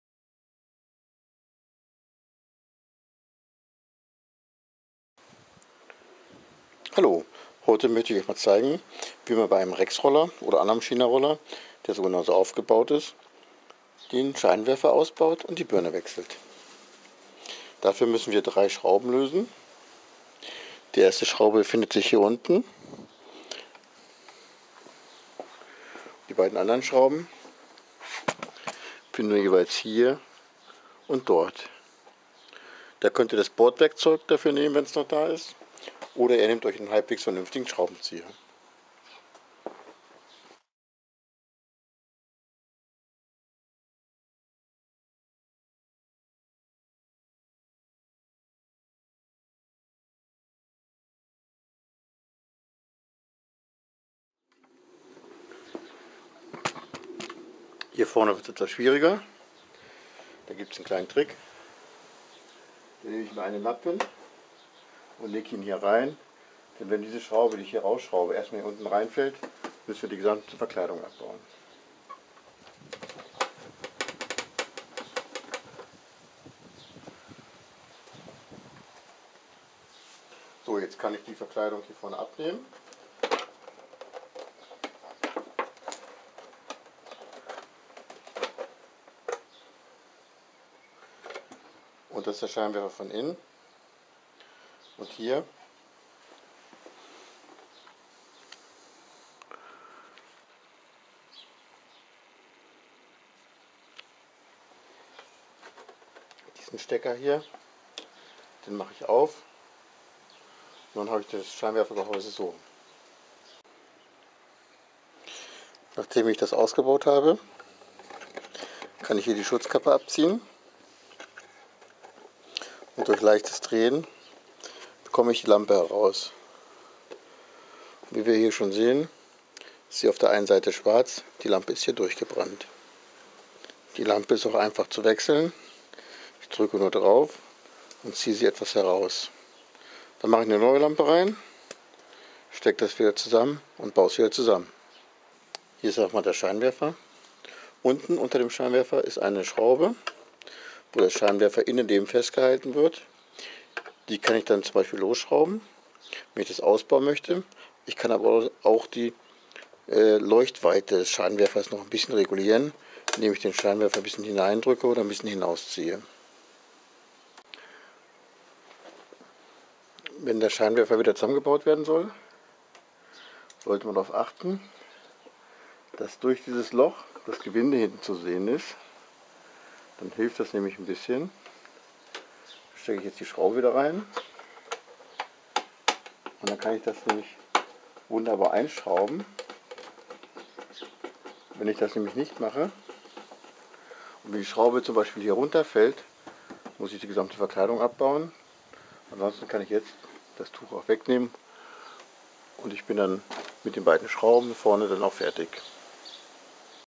Rex Roller RS400, RS450, RS460 Scheinwerferbirne Abblendlicht wechseln, Hersteller Qingqi
Rex RS 450 RS400, RS 460, OffLimit, Speedy usw. Eigentlich ganz einfach...
nur 3 Schrauben lösen und schon kann man die Scheinwerferbirne oder Lampe für das Abblendlicht wechseln